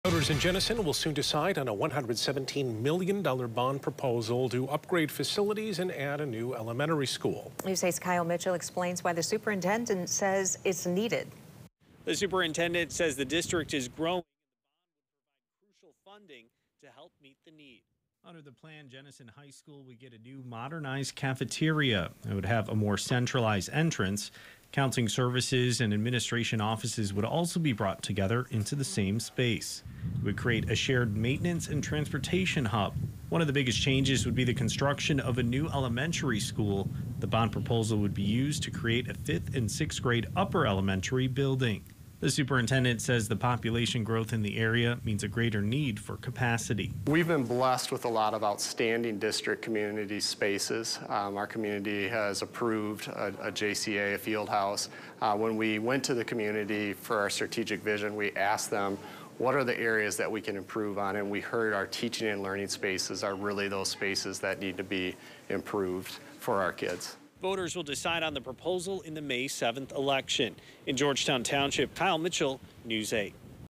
Jenison's $117 million bond proposal would pay for new school building upgrades
The May 7 election includes a $117 million bond proposal for Jenison Public Schools that, if passed by voters, would pay to build a new elementary school and modernize several facilities. (April 23, 2024)